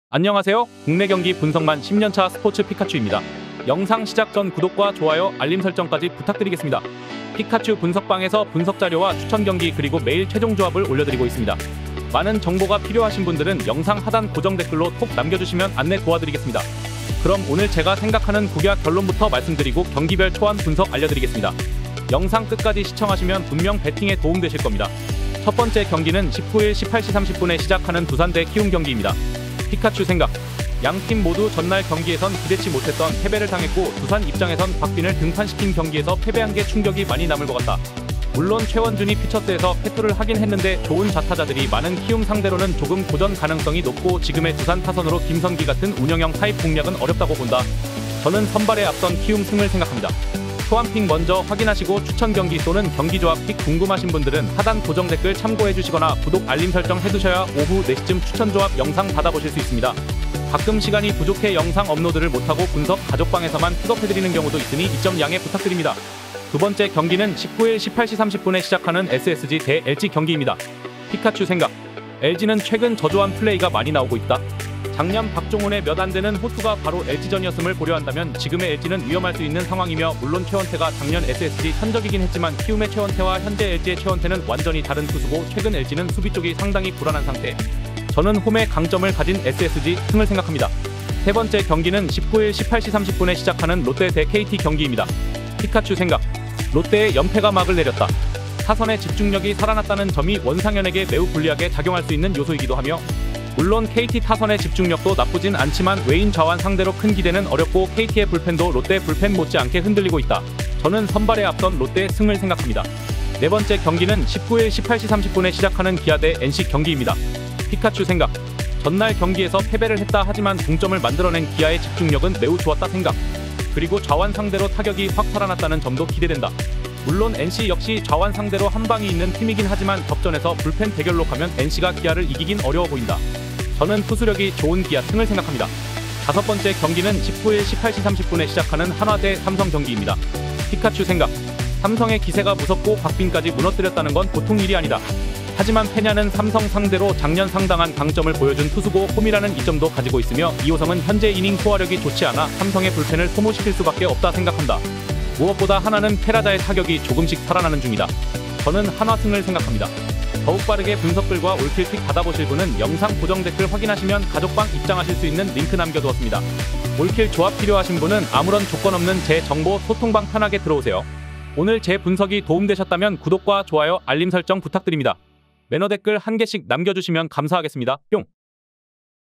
국야분석 4월19일 5경기 국내 kbo 프로야구 분석 스포츠토토 분석완료
국야 분석 4월19일 KBO 프로야구 경기 분석픽업~
경기 조합 또는 스포츠토토 분석 프리뷰, 올킬 픽 궁금하신분은
하단 고정댓글 참고하셔서 연락주시면 입장 도와드리겠습니다.
오늘도 국야 적중 달려보겠습니다.
베팅에 도움이 되셨다면 매너 댓글 부탁드리겠습니다!